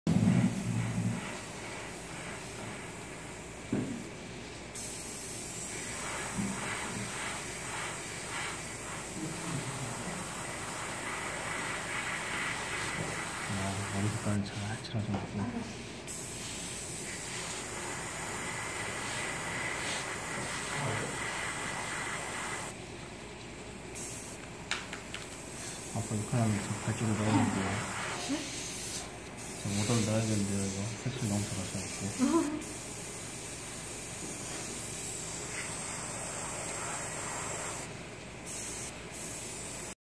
튜닝타임즈 프라모델 강좌 구로팀 OOO님의 Nendoroid Custom 넨도로이드 커스텀 My Character 자캐 그라데이션 색칠
AK플라자 구로점 넨도로이드 커스텀 수업 수강생 OOO님이 스노우미쿠 스타일로 머리 부품을 색칠해보고 싶어 하셔서 전체를 흰색으로 칠하고서 하늘색으로 그라데이션을 넣어 줬는데요, 그러고 나서 블루펄, 화이트펄, 진주펄 등을 적층하면서 반짝반짝하고 은은한 느낌이 되도록 색칠을 해줬습니다^^ 프라모델 튜닝타임즈